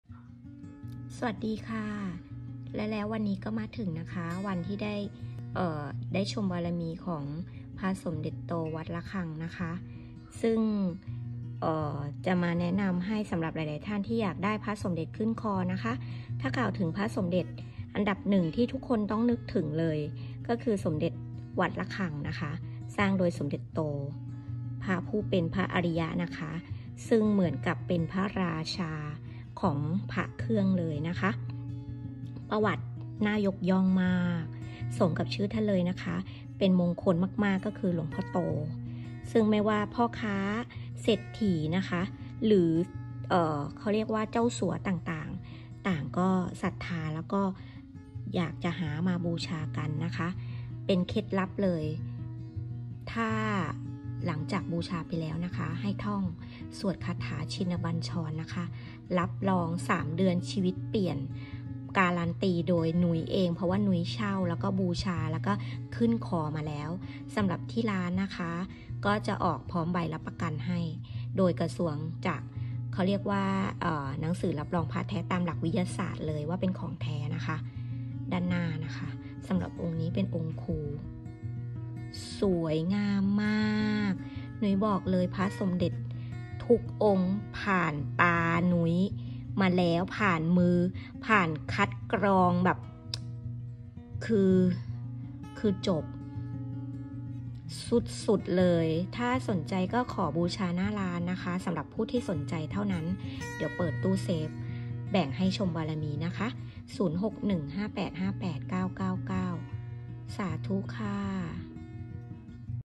แนะนำเช่าบูชาพระสมเด็จวัดระฆังที่ไหนดี แนะนำร้านสหพระเครื่อง 0615858999
วันนี้..มาแนะนำพระสมเด็จขึ้นคอค่ะ🙏
ถ้ากล่าวถึงพระสมเด็จ อันดับหนึ่งทุกคนต้องนึกถึง พระสมเด็จวัดระฆังสร้างโดยสมเด็จโตผู้เป็นพระอาริยะซึ่งเป็นราชาและจักรพรรดิ์ของพระเครื่อง บารมีแก่กล้าและประวัติน่ายกย่อง รวมกับชื่อของท่านอันเป็นมงคลคือหลวงพ่อโต ไม่ว่าจะเป็นพ่อค้าเศรษฐีล้วนแต่ศรัทธาบูชาและสวดด้วยคาถาชินบัญชร อันศักดิ์สิทธิ์มีความเชื่อว่าผู้ใดศรัทธาบูชาท่านแล้ว กิจการการงานจะเจริญเติบโต สมดั่งชื่อของท่าน วันนี้มีโอกาสได้อาราธนาพระสมเด็จจากวัดระฆัง สมเด็จแท้มีใบเซอร์💵💵